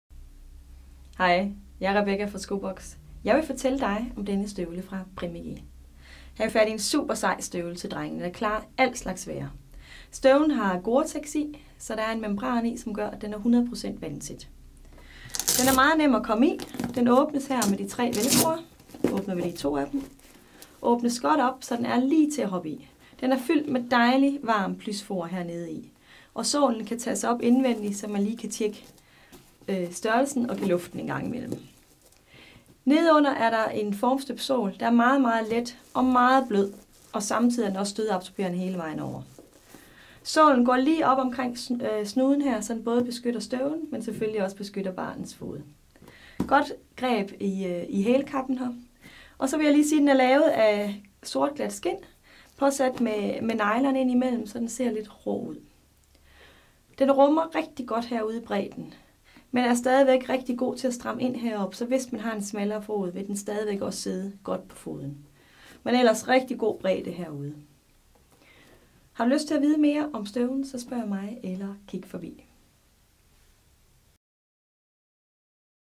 Skobox - Primigi drengestøvler 5928077 - Køb den online på skobox.dk
- Se endnu flere detaljer med 360 gradersvisningen hvor du også har mulighed for at zoome helt ind.

Hej jeg hedder Rebecca og jeg vil i denne videopræsentation fortælle dig om en Primigi støvle. Jeg gennemgår hvilke detaljer og funktioner denne Primigi drengestøvle har, således det er nemmere for dig, at se om det er de rette støvler du har valgt.